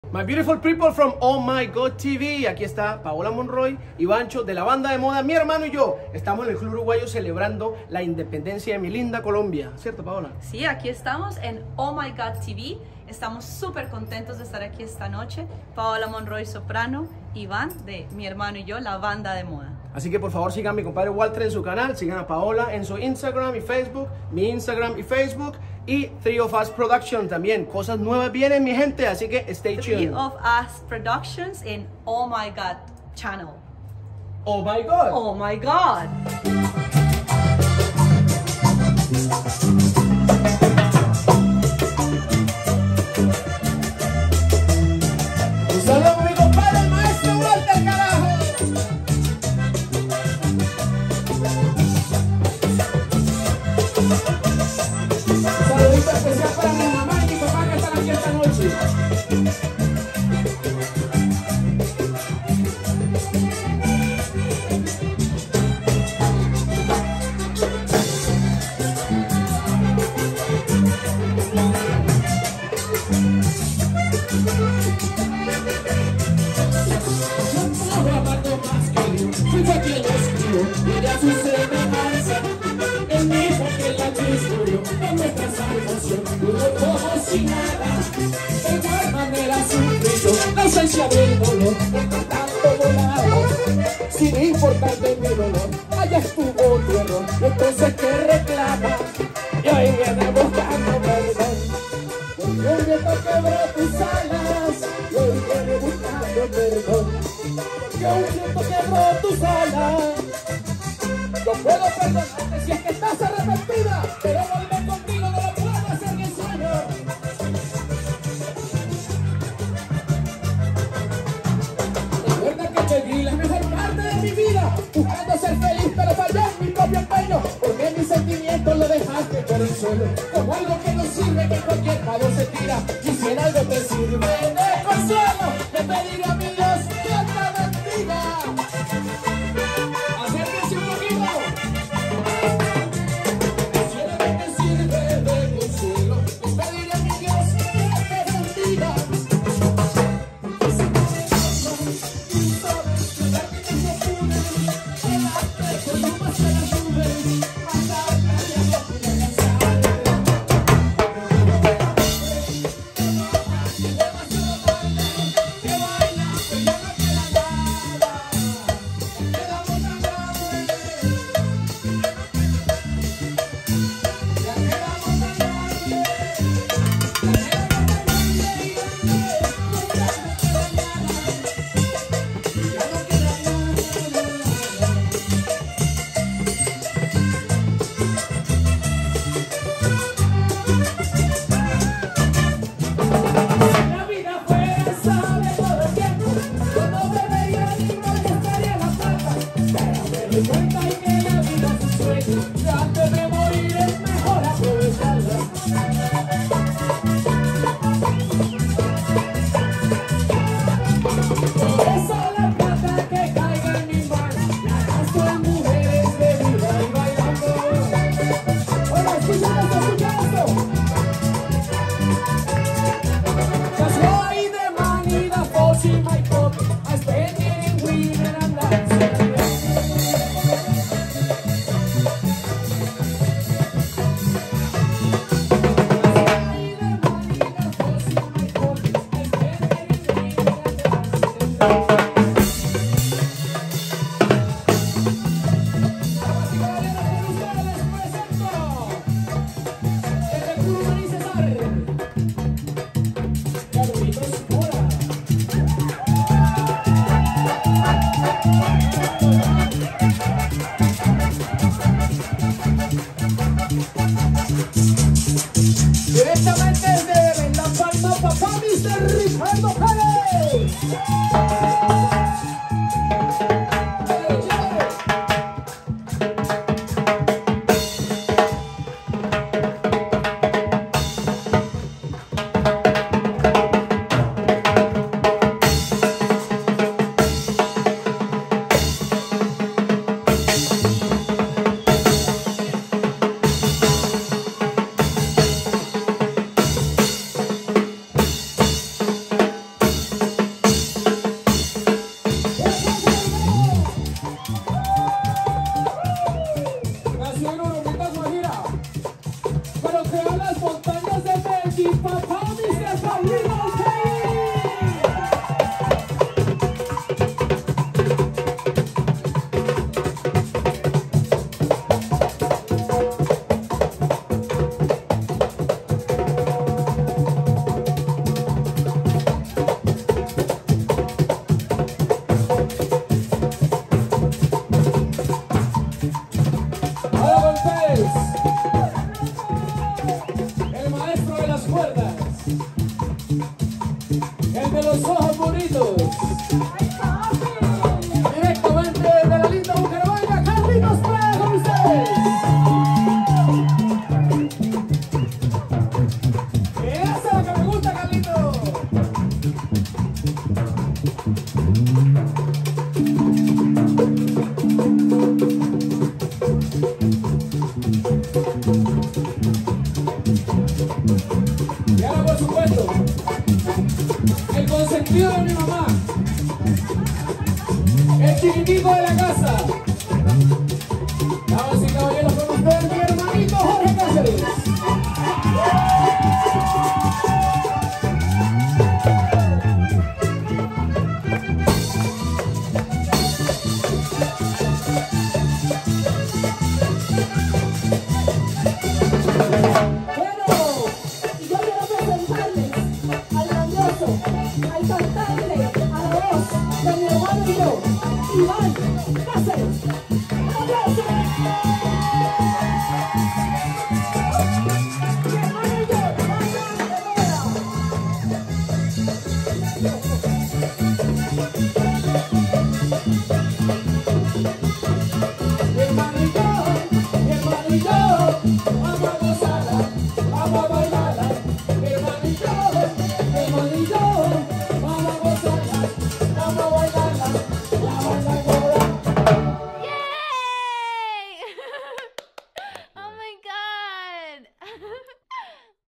MI HERMANO Y YO
Sensational live performance at the Uruguayan Club
Saturday, 22nd of July 2023
The biggest Latin Party 2023 in Celebration of The COLOMBIAN INDEPENDENCE DAY
Band: "Mi Hermano Y Yo"
Ivancho Caceres (Voz principal)
Jorge Caceres (Acordeon y Percusión)
Carlos Prada (Bajista)
Carlos Mora (Percusión)
Ricardo Perez (Timbal)
Pablo Upegui (Acordeón y Percusión)
Special guest Paola Monroy
Camera: Maestro Walter
OMG TV
Sydney - Australia MMXXIII